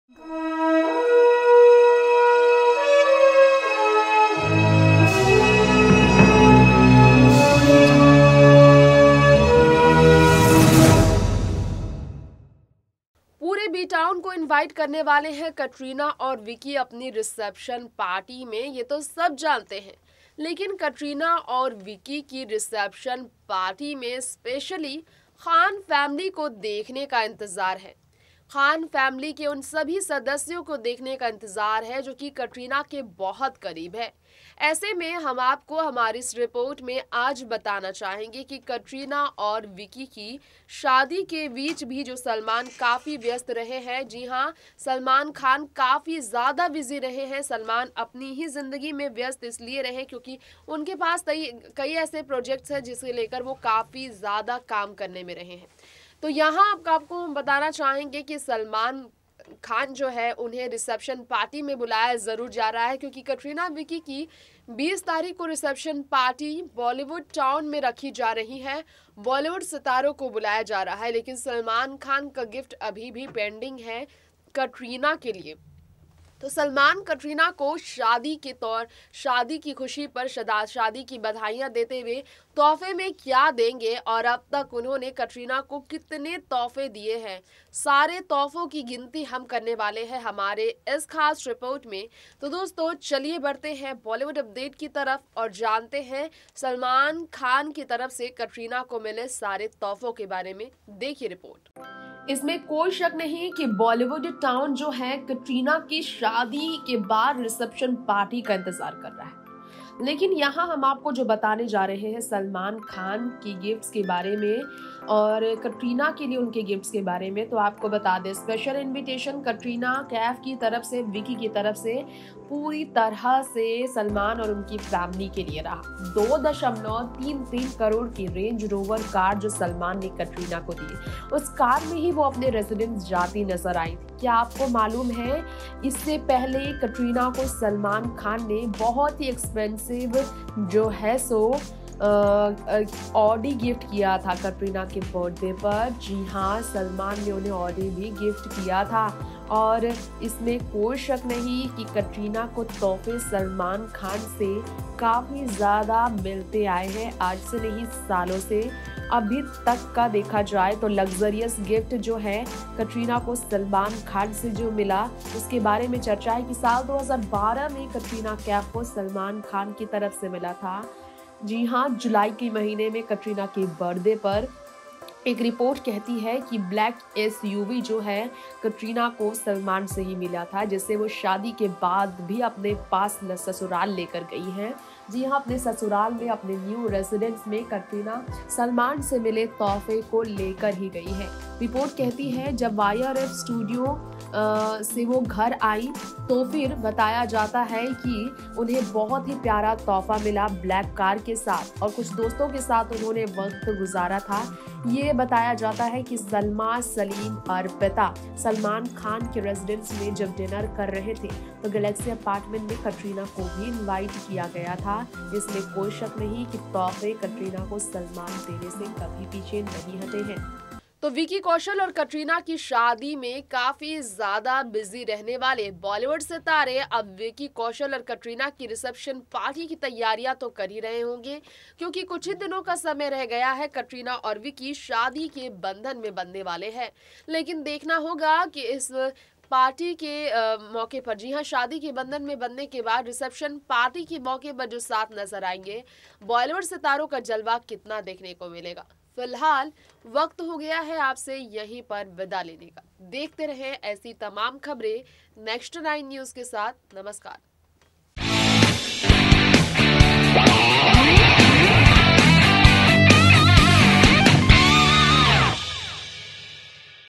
सलमान का तोहफा लेकर ससुराल पहुंची नई दुल्हन कटरीना, शादी होते ही आई ये GOOD NEWS | Salman, Katrina
जी हां ससुराल जैसे ही कटरीना कैफ पहुंची...वहां उनकी कार पर लोगो की नजर गई..जो की सलमान की तरफ से उन्हे मिला था ये बताया जा रहा हैं..